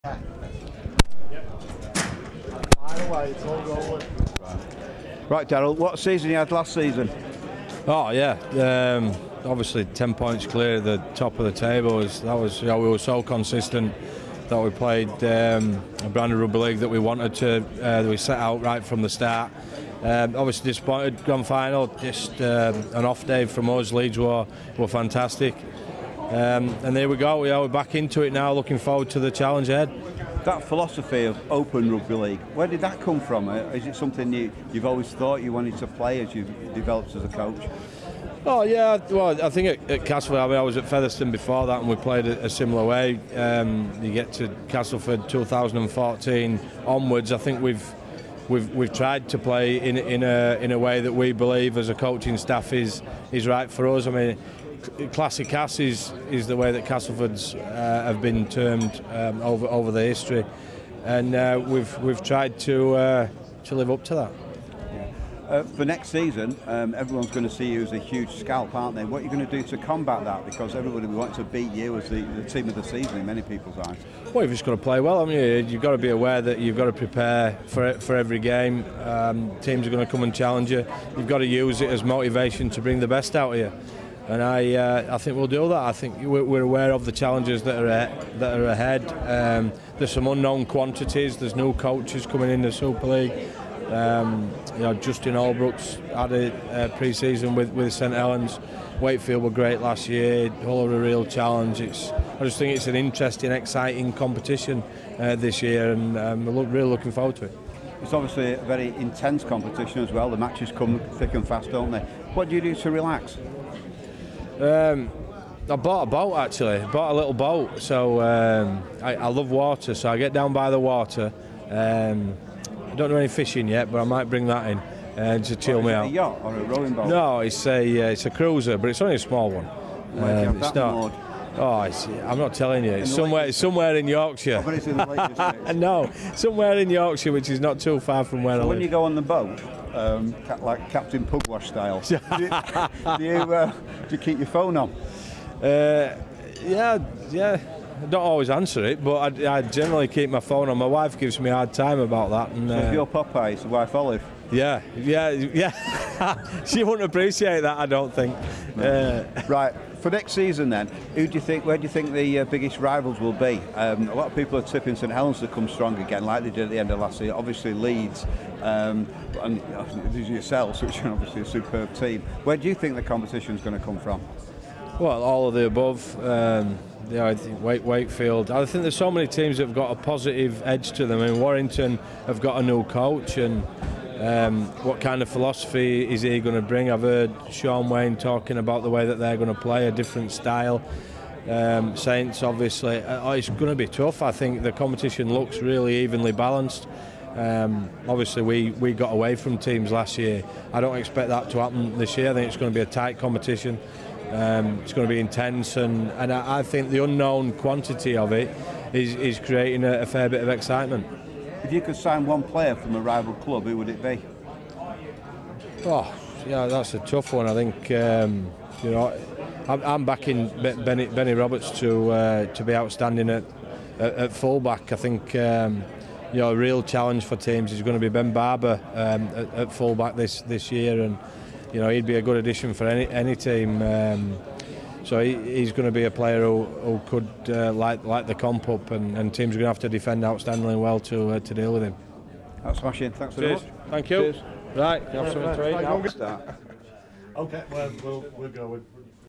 Super League Preview - Daryl Powell
Castleford Tigers coach Daryl Powell previews the new Super League season, speaking at the Betfred rugby league season launch in Huddersfield.